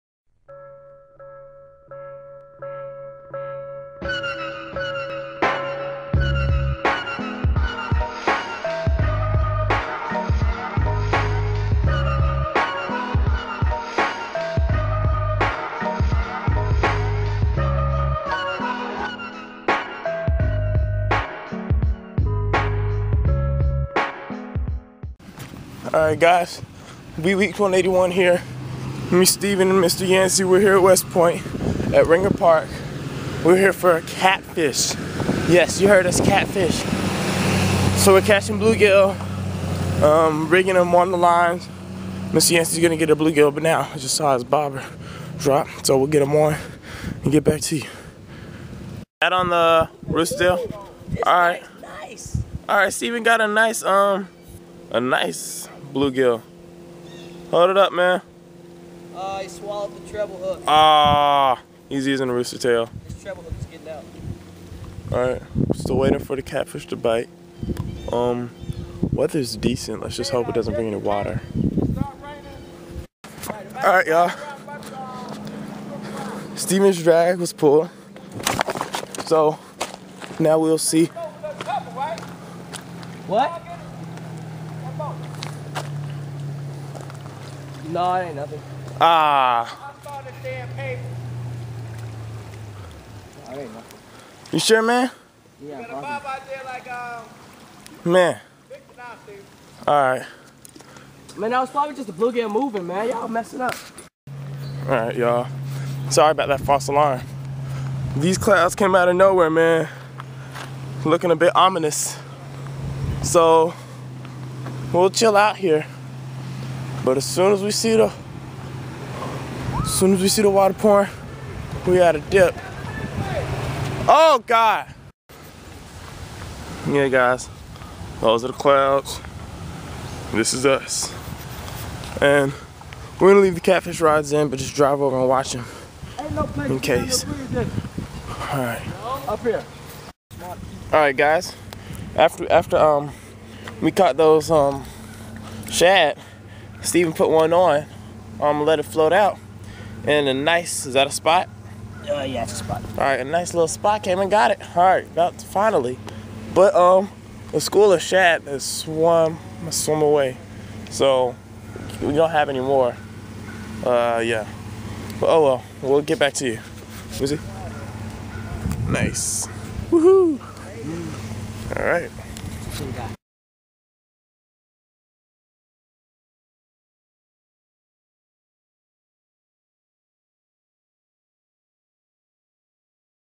West Point Scramble 5-18
Steven and I head to West Point, GA to catfish, and well, we kinda get rained out. Keep watching to see what happens!